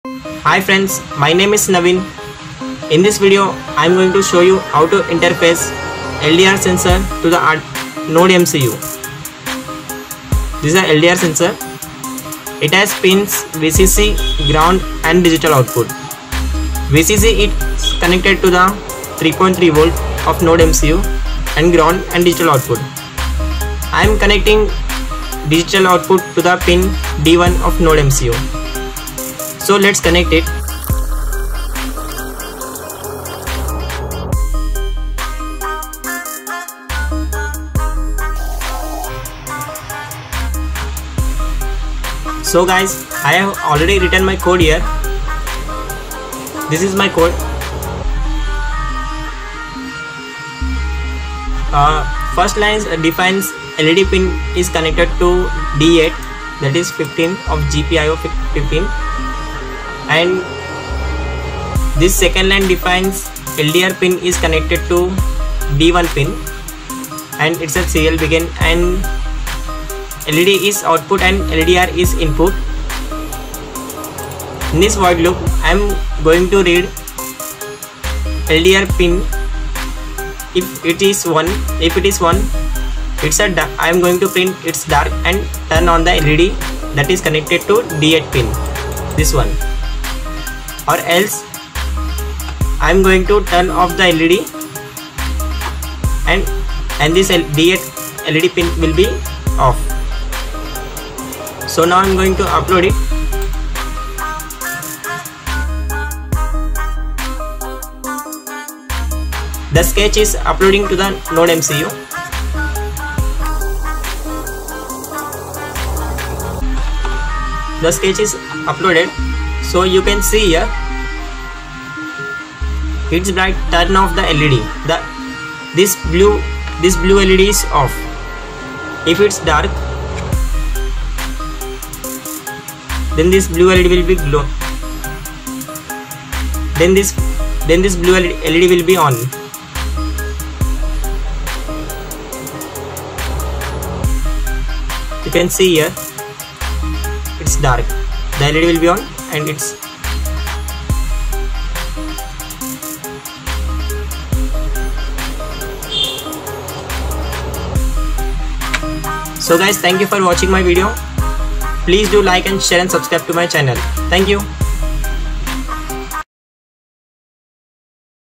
LDR Sensor With Nodemcu Arduino Code
In this video I have shown how to interface ldr Arduino sensor to nodemcu and the code will be same for the Arduino uno also only you have look changes at GPIO pins define
I have use simple led indicator, you can use relay board to implement it with AC loads

Code Link Will Be Uploaded Later Here:

keep supporting 🤗